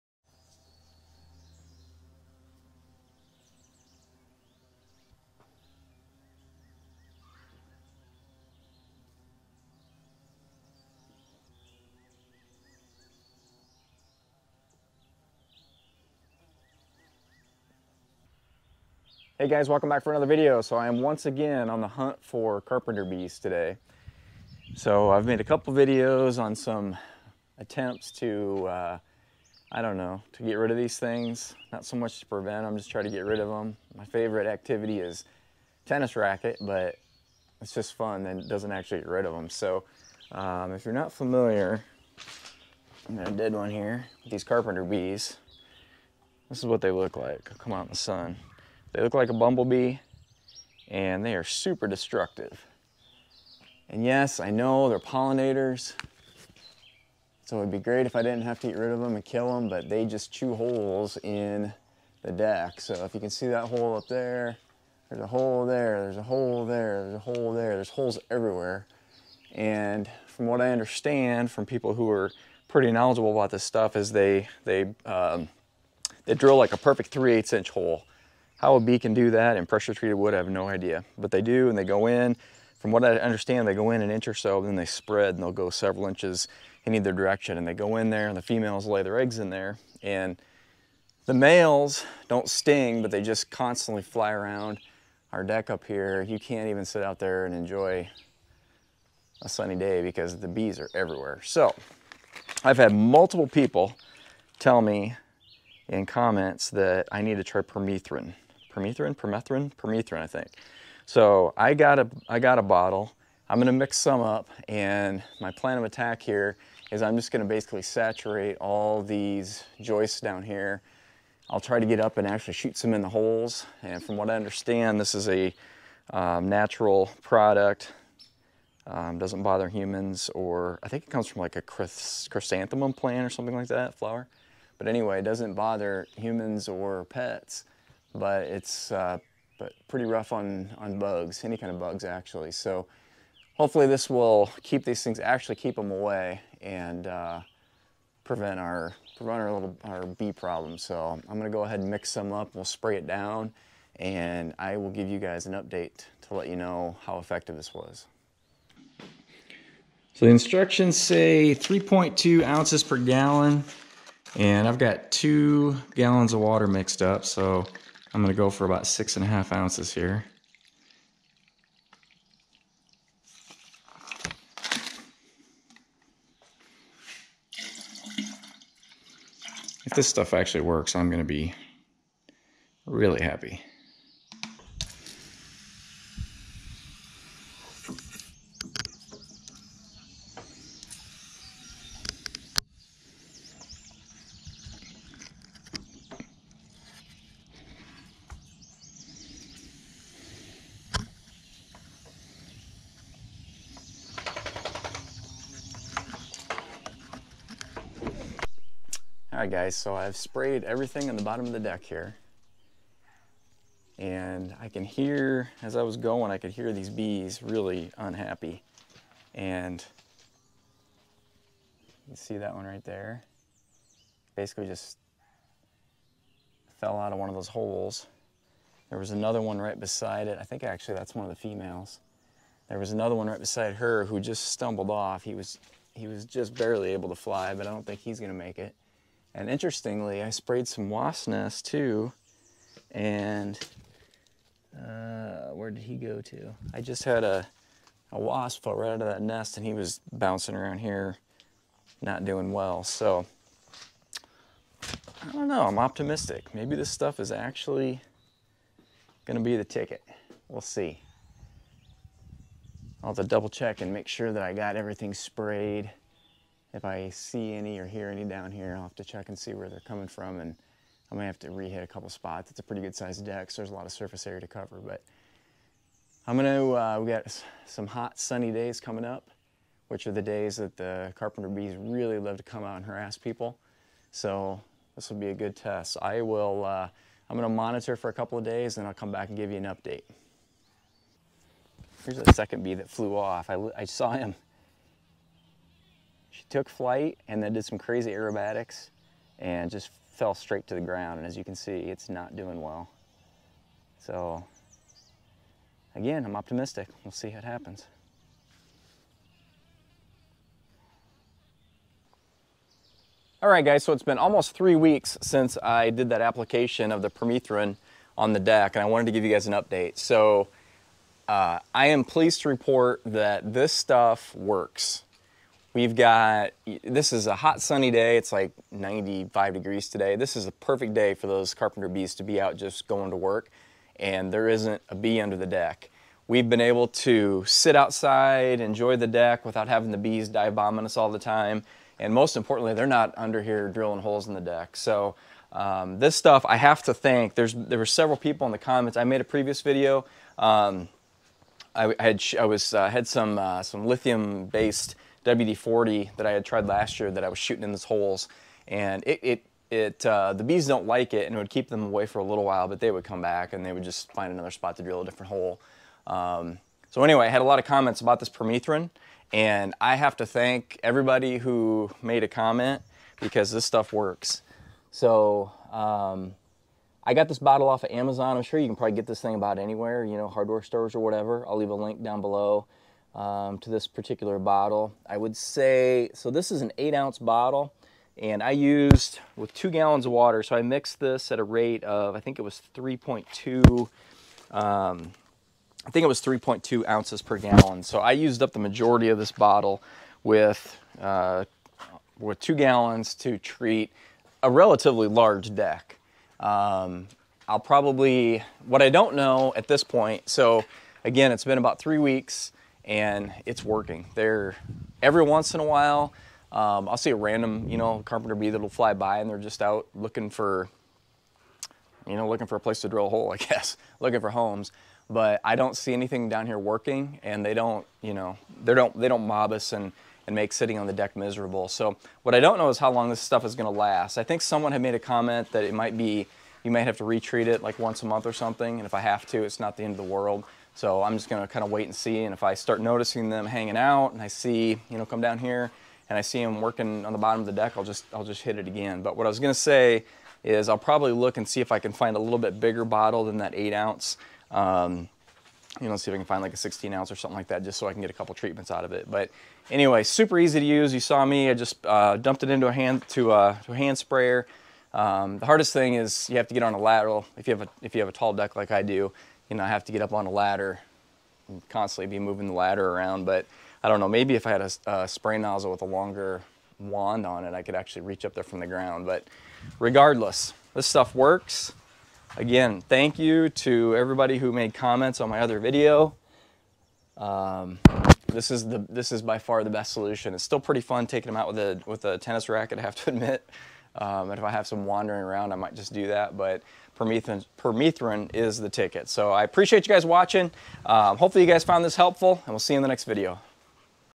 The BEST way to get rid of carpenter bees. They were destroying my deck!
The BEST way to get rid of carpenter bees!  I've tried different solutions but this is by far the most effective method I've found to kill and prevent carpenter bees.

8 0z

16oz

1 quart

Backpack Sprayer